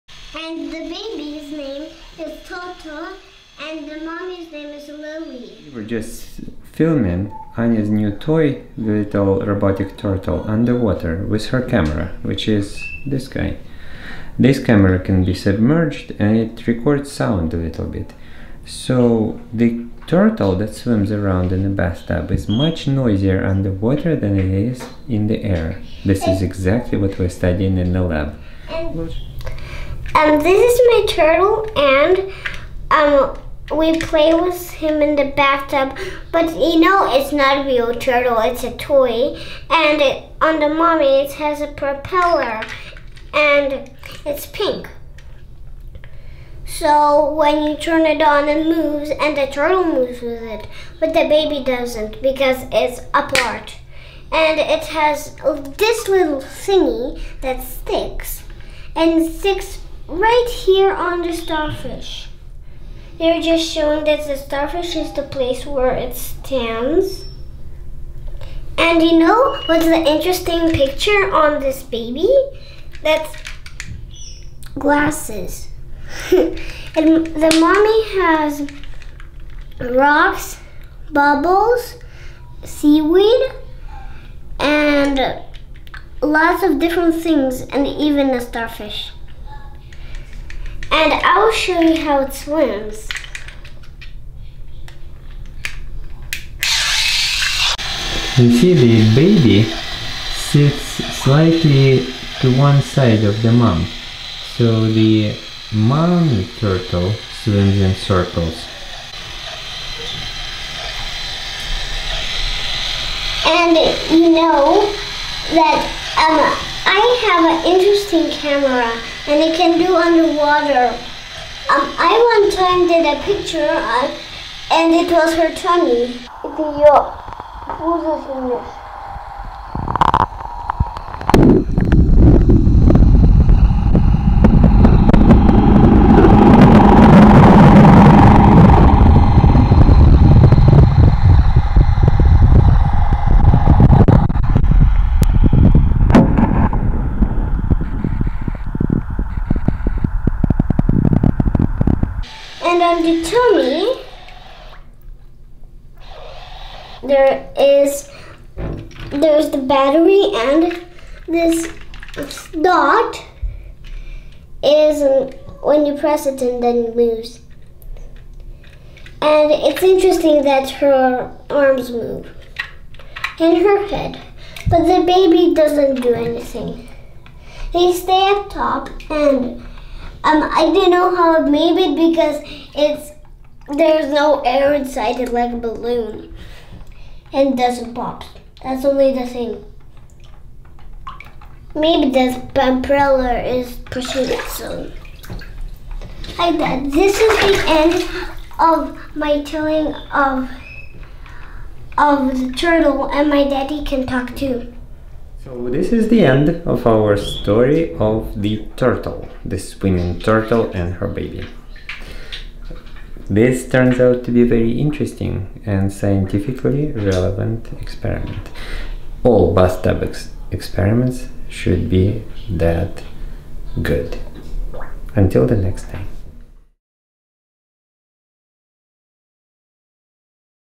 Bathtub experiments
My daughter and I recorded underwater noise of a robotic turtle toy.